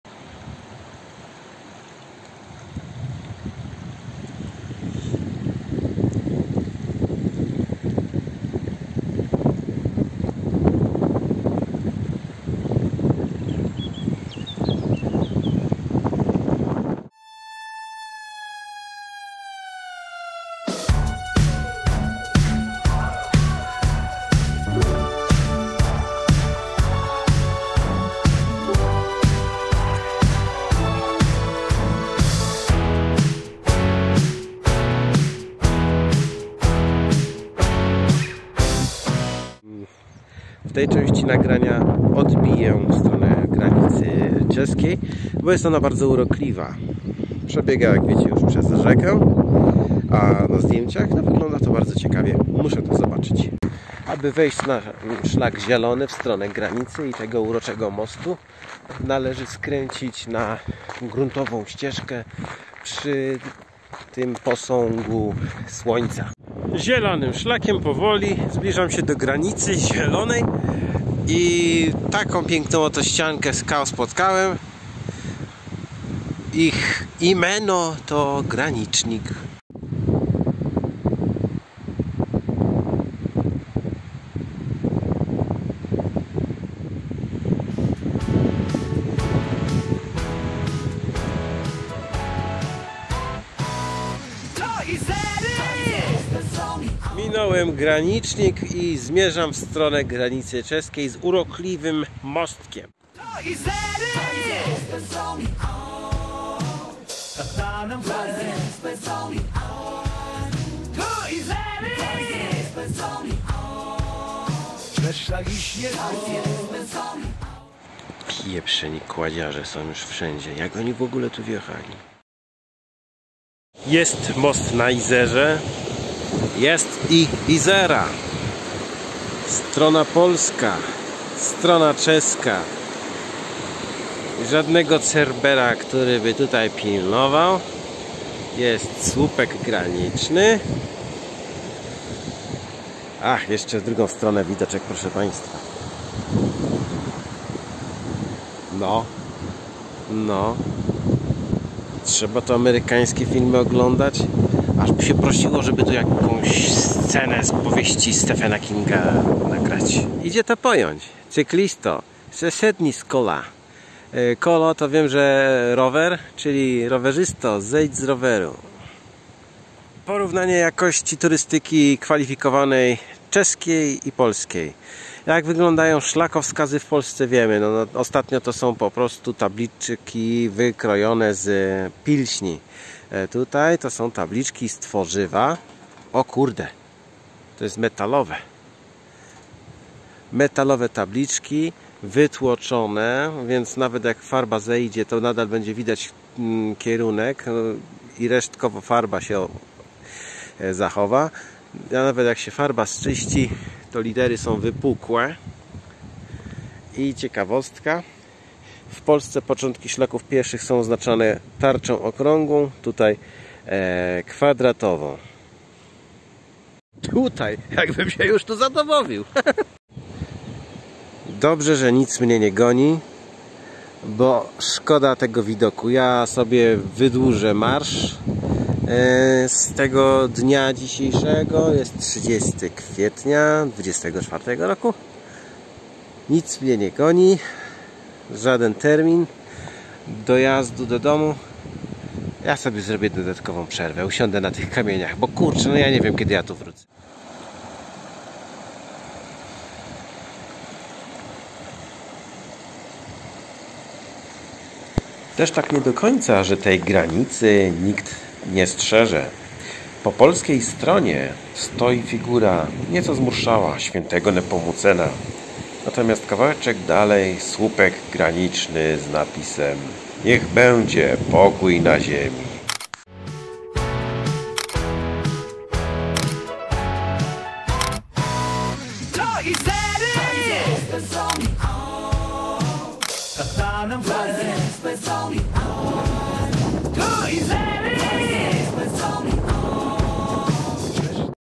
Malownicza granica państwa na Izerze. Niesamowite Izery odc.6 | Góry Izerskie
W tym odcinku ujrzycie granicę państwa pomiędzy Czechami a Polską na rzece Izera w górach Izerskich.
Sceneria jak z sielankowych powieści Stephena Kinga.
Zobaczycie także czeskie znaki turystyczne - na wypasie. No i dowiesz się, co to jest czeskie kolo po polsku.

Napisy:

NIESAMOWITE IZERY - odc.6

realizacja
Dominik Wójcik

zdjęcia zrealizowano w dniach
29 kwietnia - 1 maja 2024